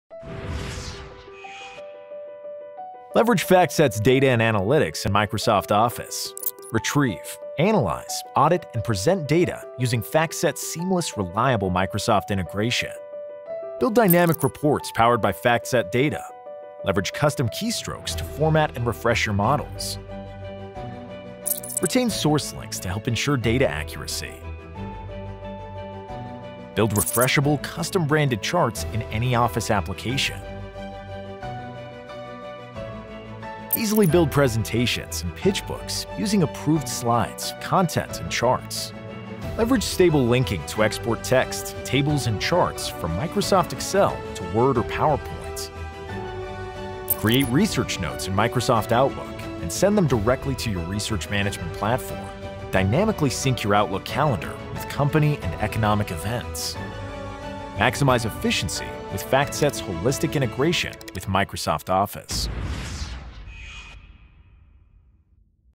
FactSet | Microsoft Office Integration
Leverage FactSet’s data and analytics in Microsoft Office. Retrieve, analyze, audit, and present data using FactSet’s seamless, reliable Microsoft integration.